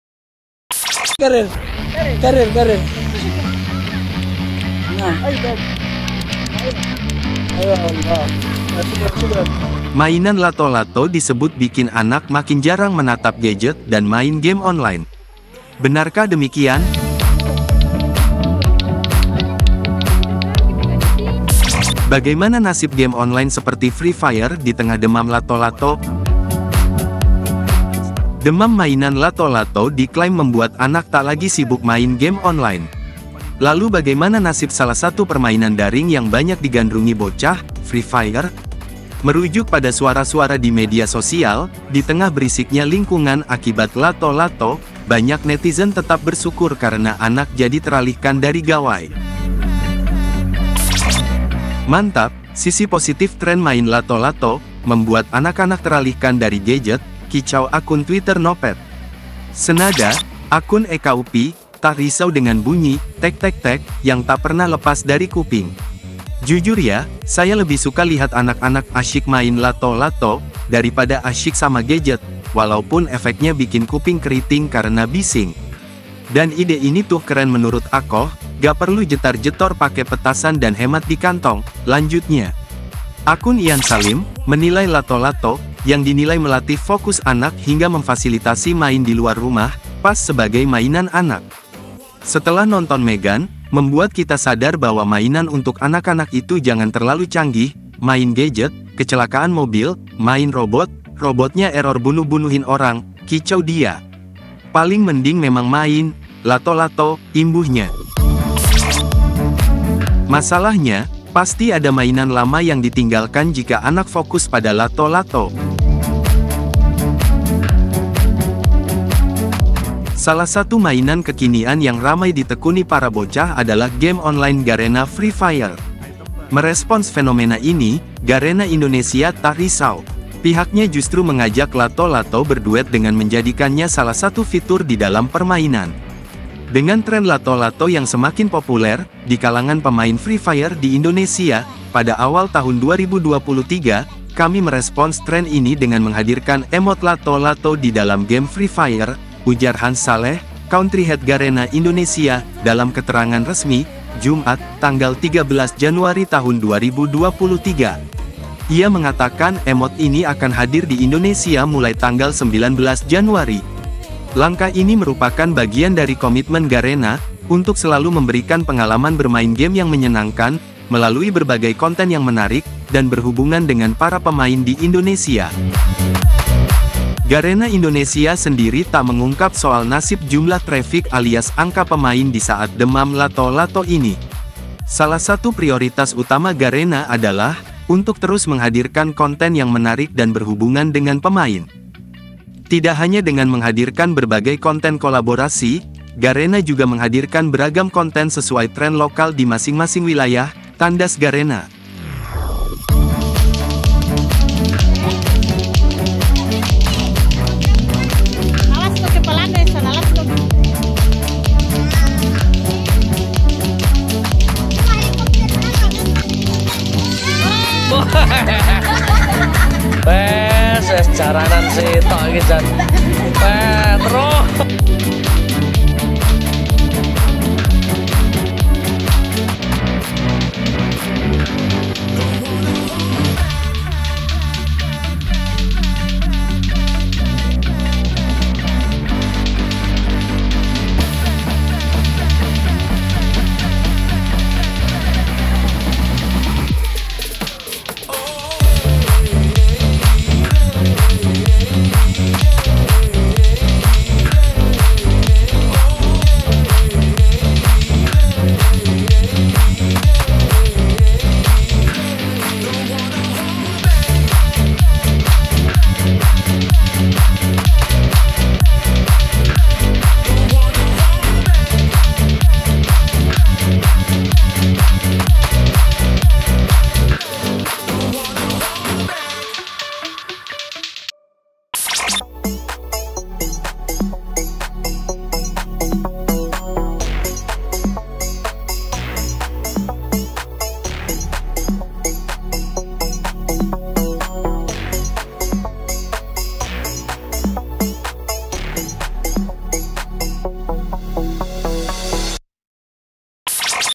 Lato lato Bikin Bocah Tinggalkan Game Online Free Fire Buka Suara INA ON TRend
Lato-lato Bikin Bocah Tinggalkan Game Online? Free Fire Buka Suara.

Mainan lato-lato disebut bikin anak makin jarang menatap gadget dan main game online. Benarkah demikian?Ilustrasi. Bagaimana nasib game online seperti Free Fire di tengah demam lato-lato? (dok. Garena)

Demam mainan lato-lato diklaim membuat anak tak lagi sibuk main game online.
Lalu bagaimana nasib salah satu permainan daring yang banyak digandrungi bocah, Free Fire?

"mantap, Sisi positif trend main lato-lato, membuat anak-anak teralihkan dari gadget," kicau akun Twitter @noped.

Senada, @EkaUpi : "tak risau dengan bunyi "tektektek" yang tak pernah lepas dari kuping."

"Jujur ya, saya lebih suka lihat anak anak asyik main lato lato, daripada asyik sama gadget, walaupun efeknya bikin kuping keriting karena bising."

"Dan ide ini tuh keren menurut akoh, gak perlu jetar jetor pake petasan dan hemat dikantong," lanjutnya.

Akun @IanSalim, menilai lato-lato, yang dinilai melatih fokus anak hingga memfasilitasi main di luar rumah, pas sebagai mainan anak.

"Setelah nonton MeGAN, membuat kita sadar bahwa mainan untuk anak-anak itu jangan terlalu canggih: Main gadget, kecelakaan mobil... main robot, robotnya error bunuh2in orang," kicau dia.

"Paling mending memang main. lato lato," imbuhnya.

Masalahnya, pasti ada mainan lama yang ditinggalkan jika anak fokus pada lato-lato. Salah satu mainan kekinian yang ramai ditekuni para bocah adalah game online Garena Free Fire.

Merespons fenomena ini, Garena Indonesia tak risau. Pihaknya justru mengajak lato-lato berduet dengan menjadikannya salah satu fitur di dalam permainan.

"Dengan tren lato-lato yang semakin populer, di kalangan pemain Free Fire di Indonesia, pada awal tahun 2023, kami merespons tren ini dengan menghadirkan emote lato-lato di dalam game Free Fire," ujar Hans Saleh, Country Head Garena Indonesia, dalam keterangan resmi, Jumat (13/1/2023).

Ia mengatakan emote ini akan hadir di Indonesia mulai 19 Januari.

"Langkah ini merupakan bagian dari komitmen Garena, untuk selalu memberikan pengalaman bermain game yang menyenangkan, melalui berbagai konten yang menarik, dan berhubungan dengan para pemain di Indonesia."

Garena Indonesia sendiri tak mengungkap soal nasib jumlah traffic alias angka pemain di saat demam lato-lato ini.

"Salah satu prioritas utama Garena adalah, untuk terus menghadirkan konten yang menarik dan berhubungan dengan pemain. Tidak hanya dengan menghadirkan berbagai konten kolaborasi, Garena juga menghadirkan beragam konten sesuai tren lokal di masing-masing wilayah," tandas Garena.

Channel ini memuat Seputar Informasi yang lagi ngetrend dan Hits di Indonesia, Manca Negara dan Seluruh Alam Semesta  ^_^
Silahkan Tinggalkan Jejak Anda disini.!!!